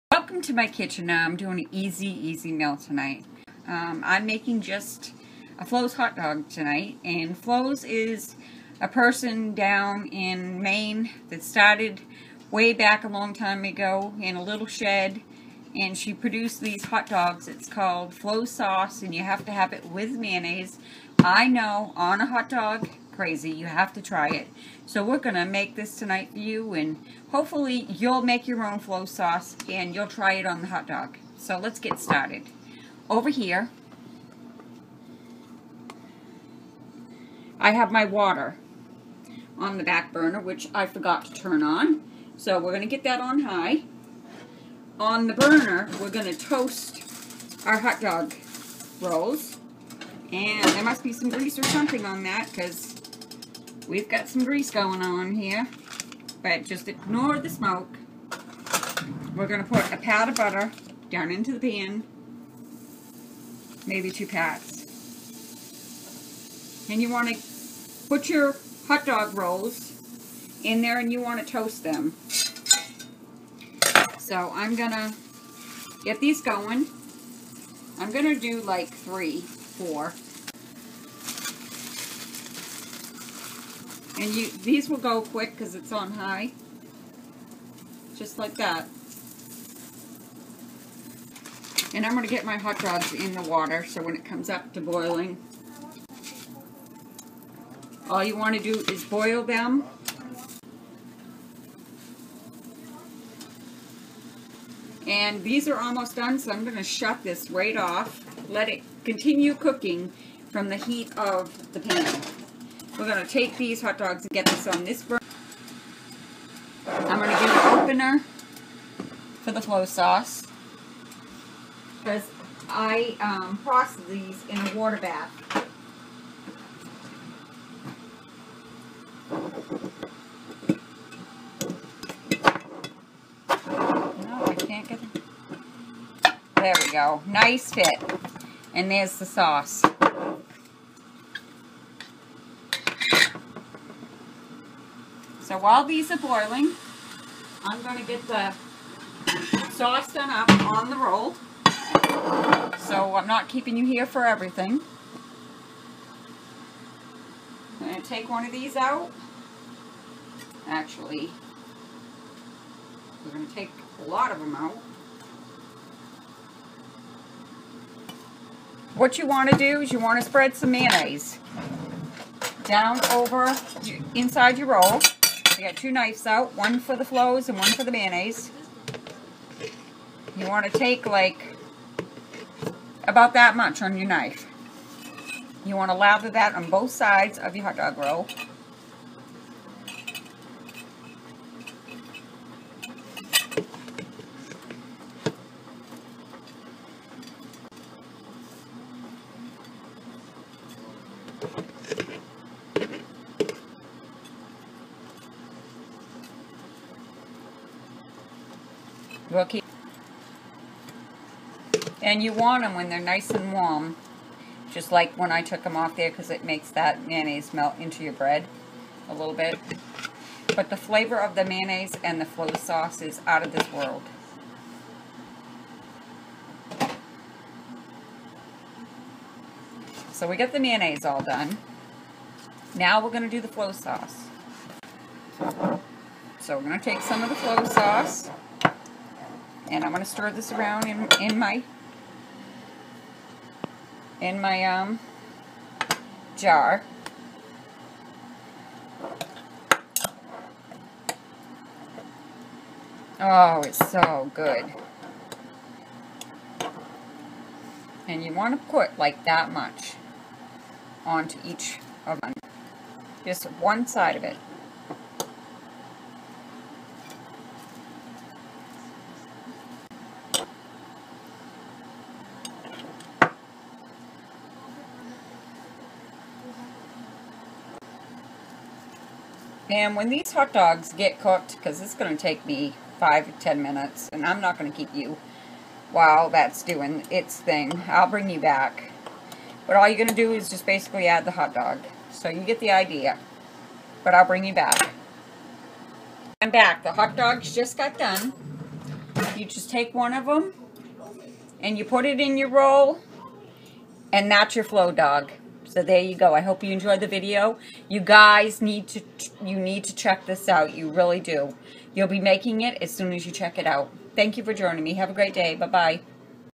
Flo's Hot Dogs-is what is for supper!
I thought i had a Video of the sauce next time I make it I will defiantly Post the Video:

Flo's Hot Dog Sauce
Ingredients:

Yield:
10-11
cups Units: US | Metric
10 lbs yellow onions, chopped fine
1 quart molasses, Sugar Mill
1 cup white vinegar
1 lb brown sugar
1 1/2 tablespoons crushed red pepper flakes
1/2 ounce Frank's red hot sauce
Directions:

1
Put onions, and crushed red peppers in food processor or blender. Chop fine.
2
Put all ingredients in a big kettle, and heat until it gets hot, stirring as you go.
3
Simmer for 8 hours.
4
Put in glass jars, and store in fridge, or process in canning jars, following canning procedures.